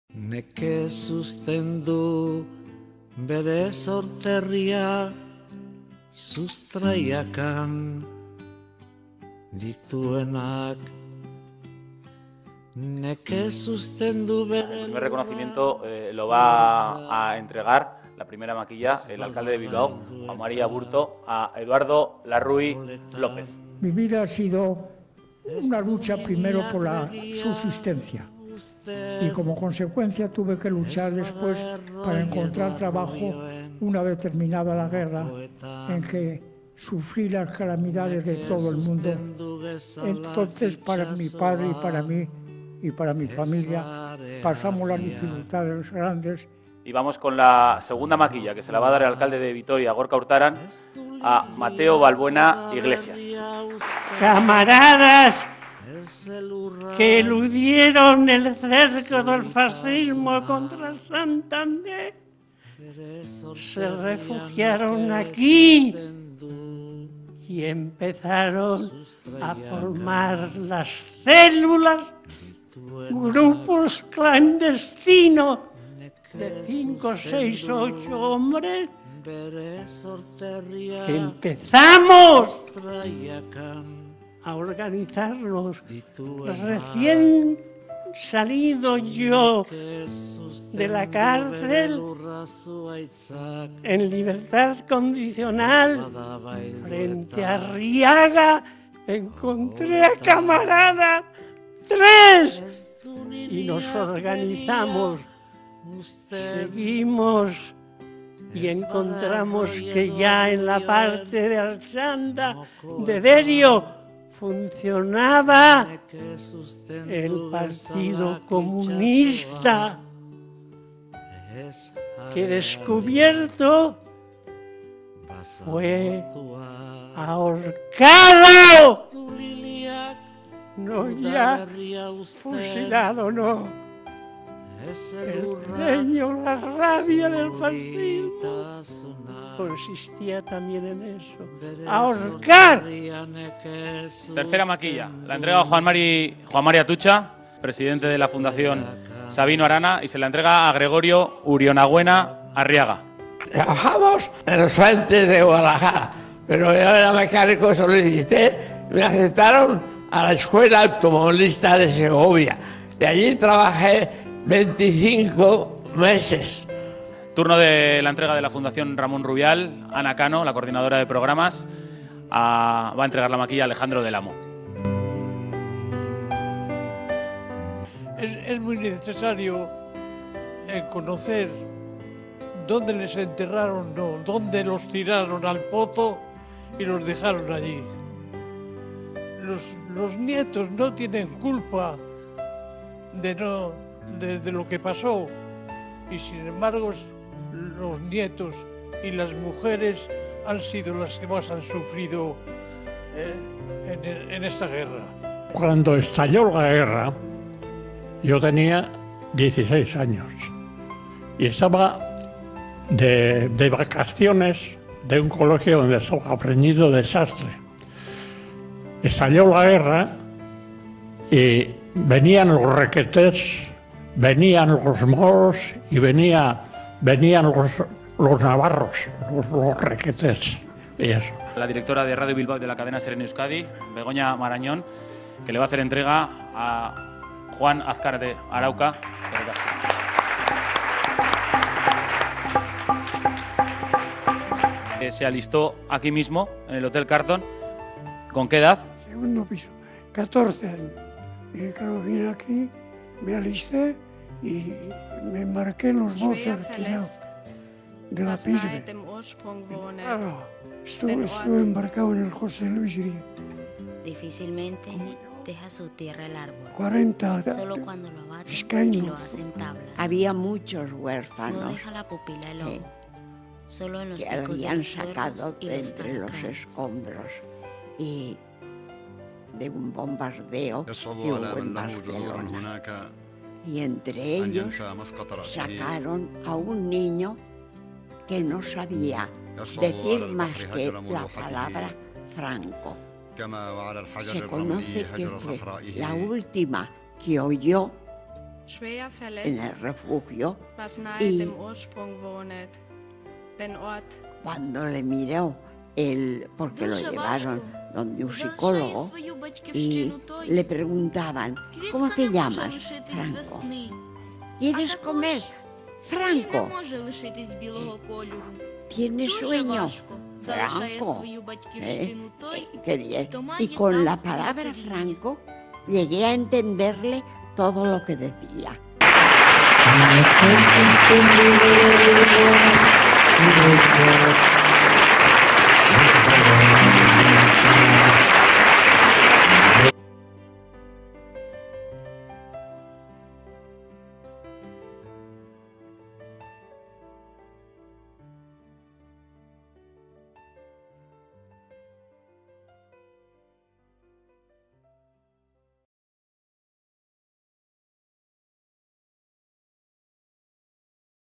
Los Últimos Vascos del Ejército Republicano
Los Gudaris fueron los hombres que formaron parte del ejército vasco durante la Guerra Civil y que lucharon contra las tropas franquistas. Los Gudaris defendieron los ideales de la República, el PNV y de Euskadi.